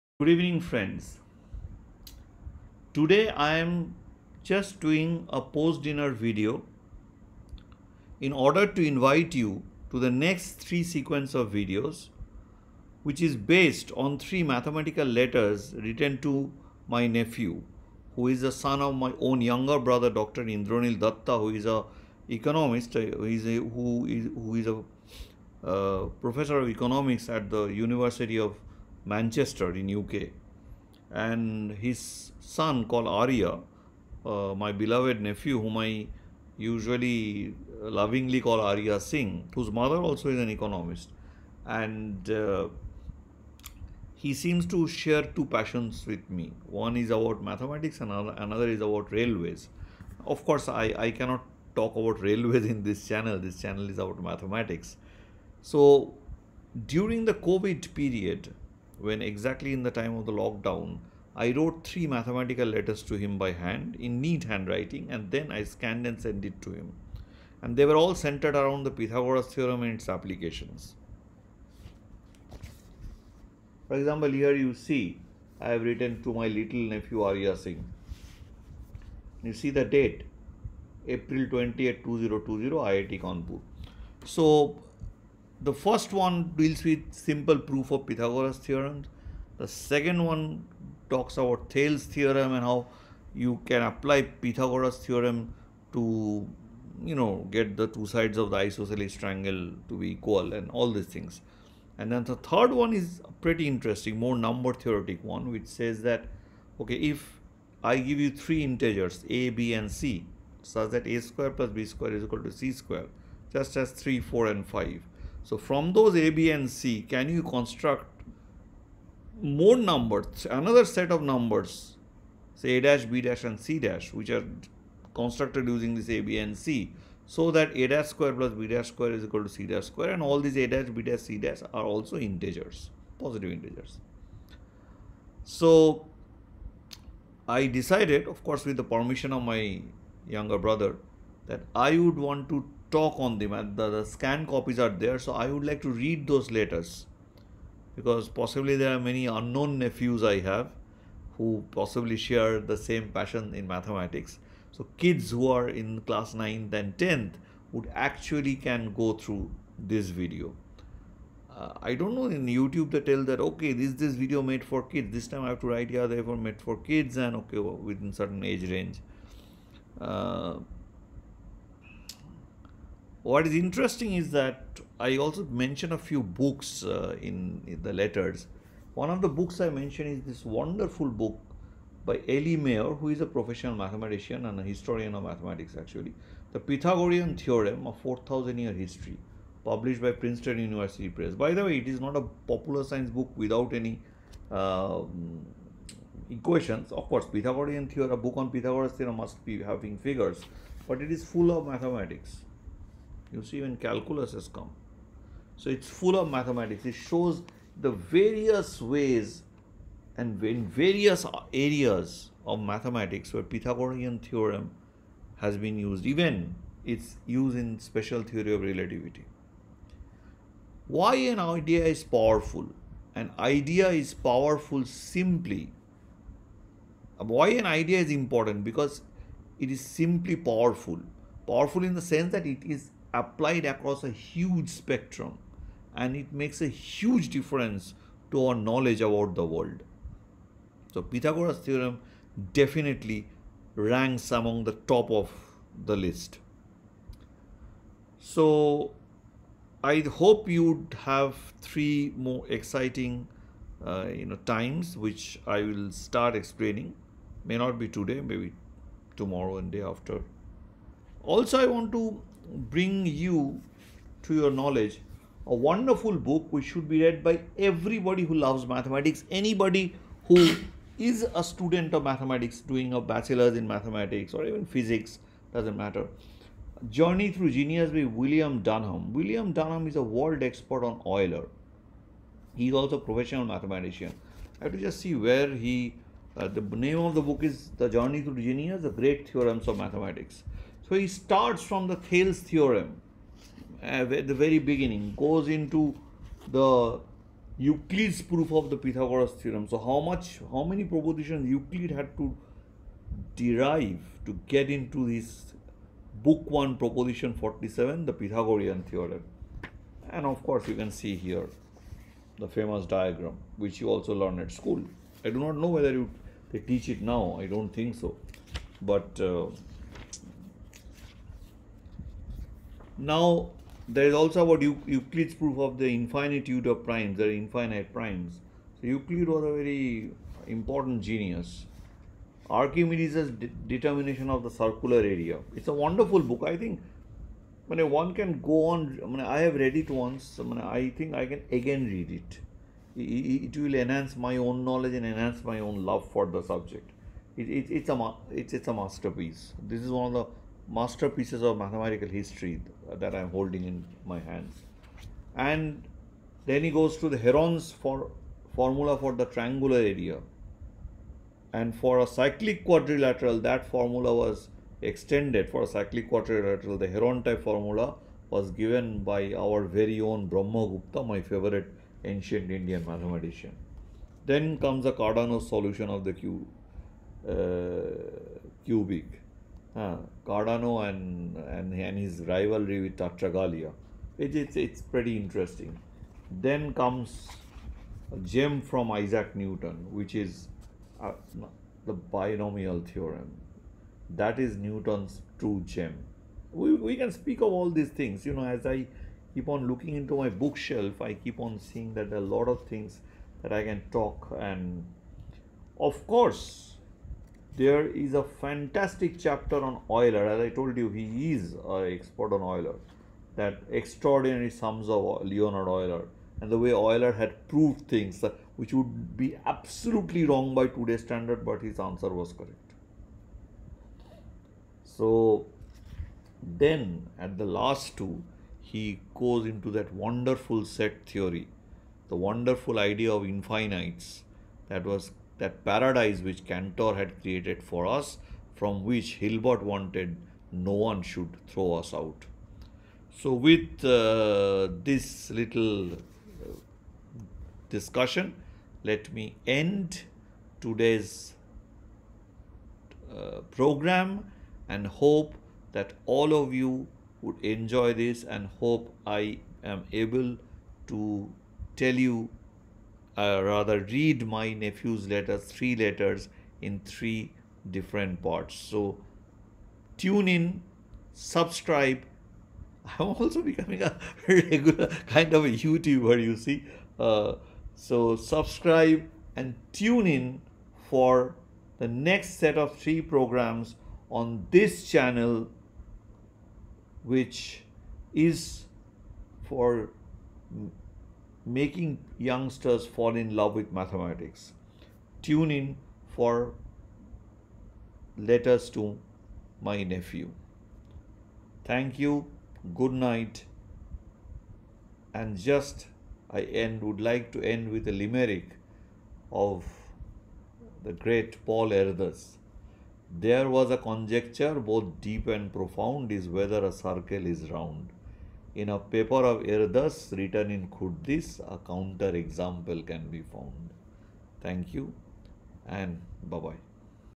Invitation to new program : Letters to my Nephew
In this video we would introduce the viewers to two books and three upcoming episodes based on letters written to my nephew during the covid lockdown. The three letters have a central theme : The Pythagoras Theorem.